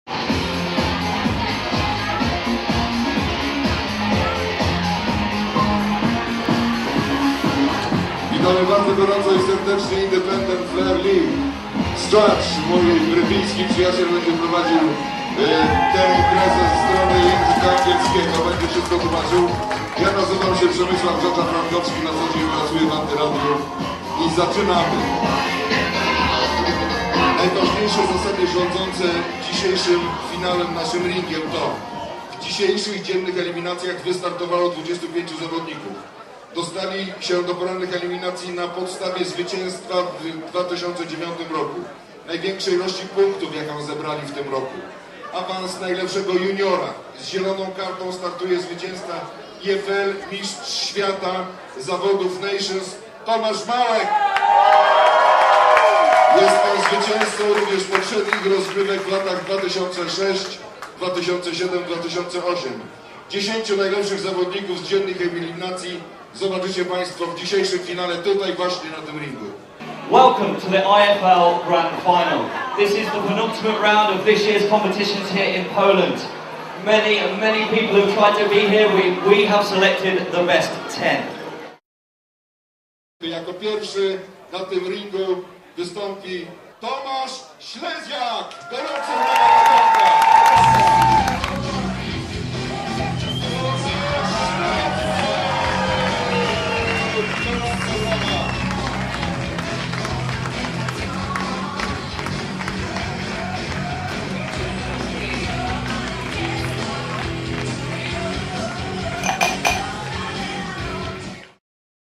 IFL Grand Final 2009 - INTRO (22nd of November 2009)
INTRO from Independent Flair League Grand Final 2009 (22nd of November 2009) Champions Restaurant Hotel Marriott, Warsaw (Poland)